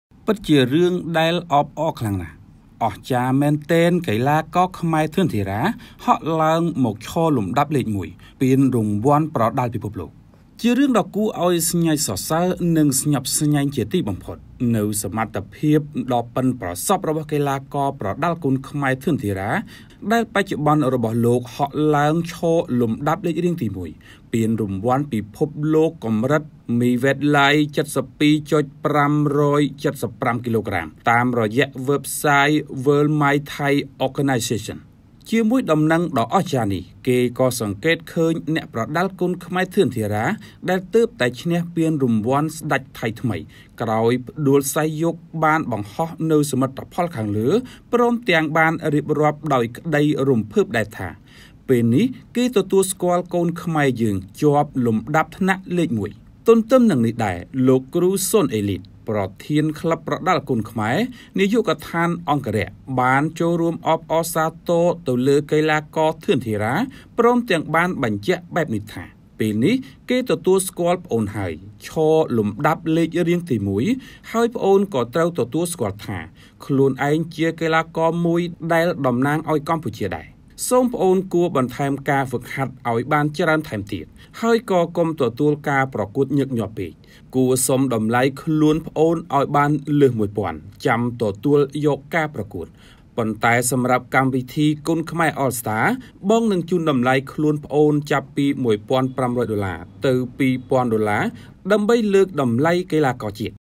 ពិតជាដំណឹងមួយគួឲ្យភ្ញាក់ផ្អើល ធឿន ធារ៉ា ពិតជាធ្វើបានមែន!
អស្ចារ្យមែនកីឡាកខ្មែរ!​ ធឿន ធារ៉ា​ ហក់ឡើងមកឈរលំដាប់លេងរៀងទី១ ក្នុងកម្មវិធីប្រដាល់ពានរង្វាន់ពិភពលោក..